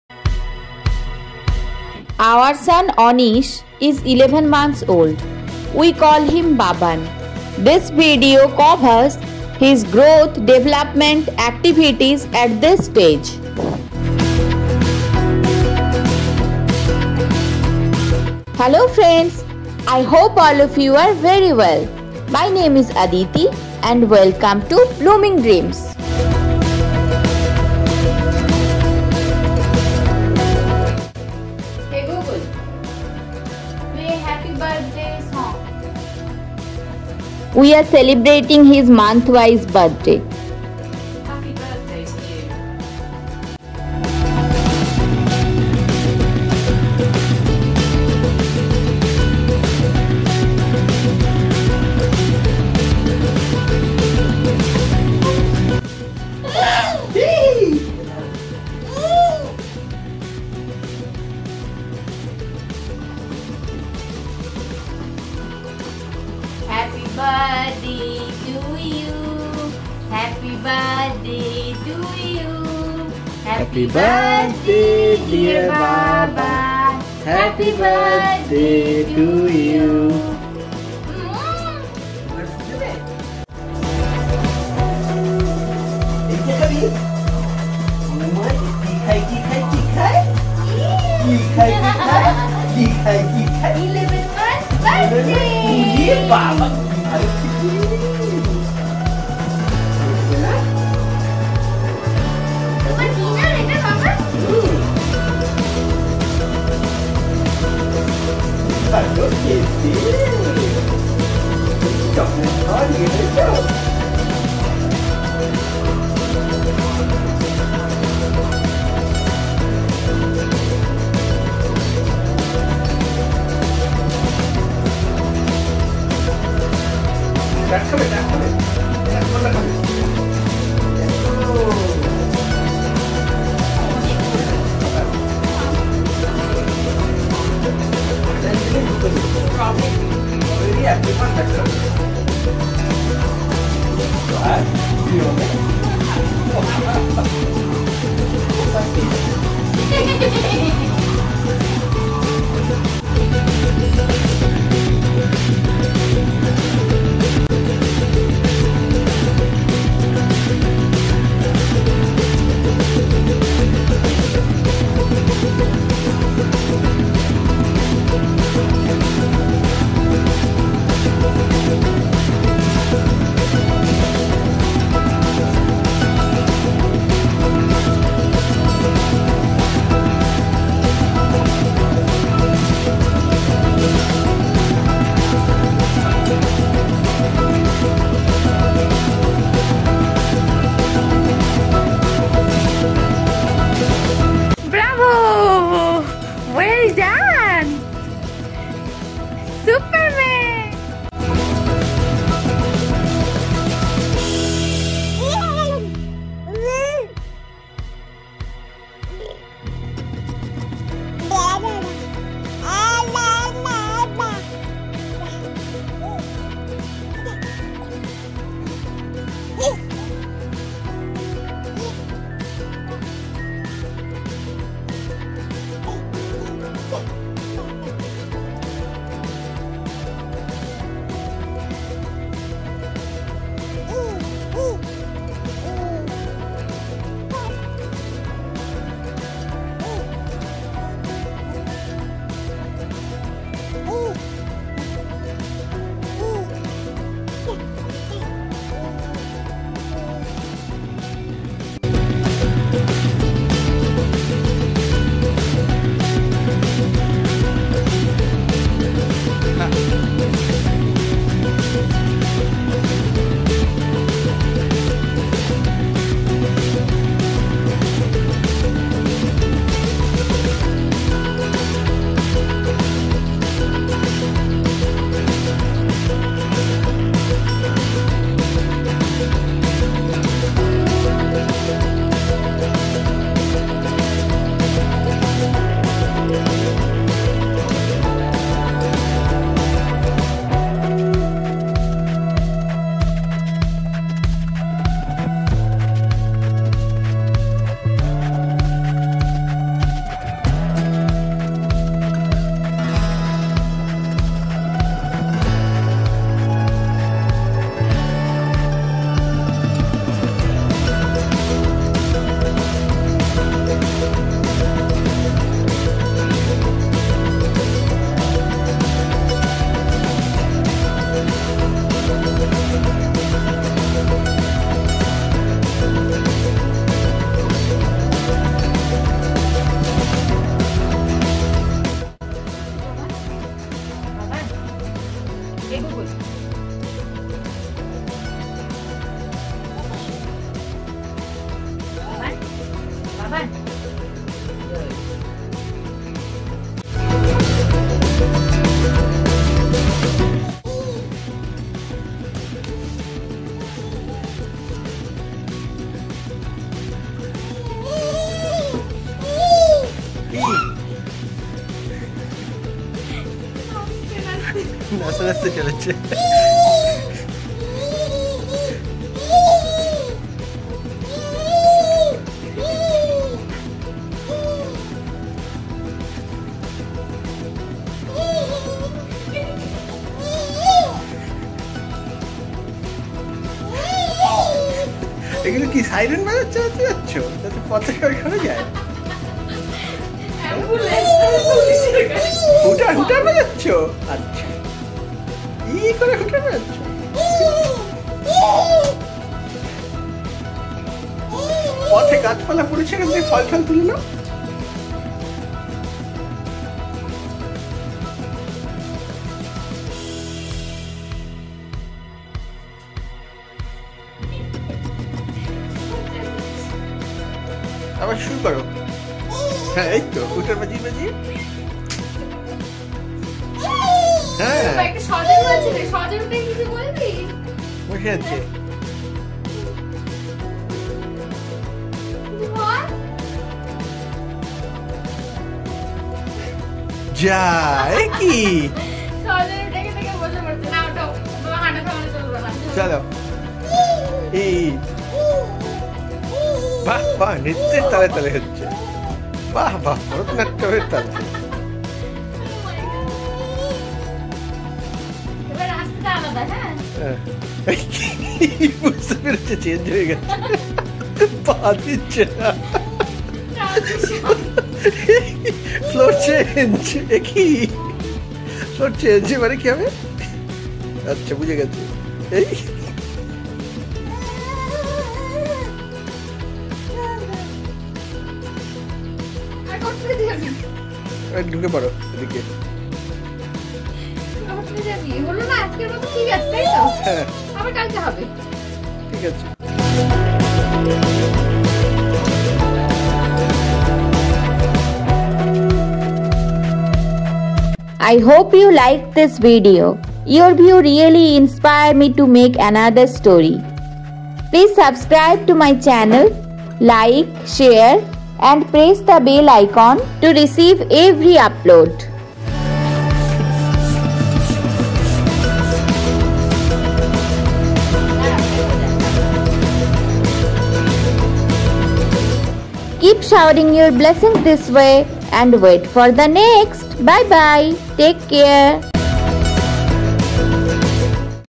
11 Months Old baby Activities I Baby Growth / Development / Milestone I @bloomingdreamz I V-39
This video contains our 11 Months old son's activities, growth, development and milestone.

0:00 Activities of our 11 months old son Anish